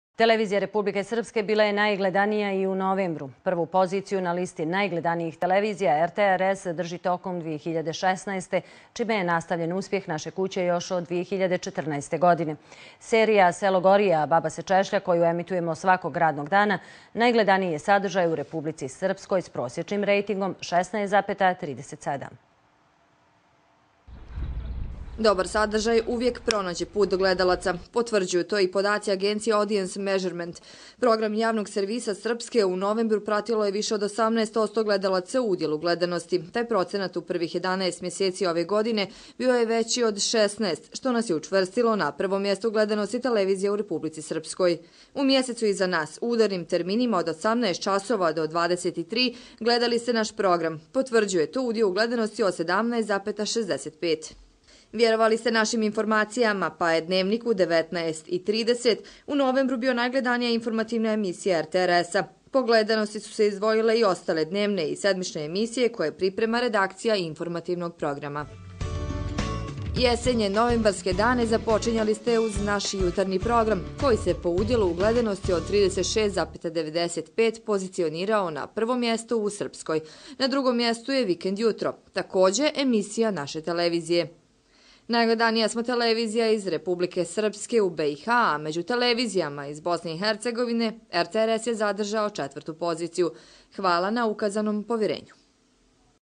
RTRS najgledaniji i u novembru
Televizija Republike Srpske bila je najledanija i u novembru. Prvu poziciju na listi najgledanijih televizija RTRS drži tokom 2016.godine, čime je nastavila trend započet još 2014. godine.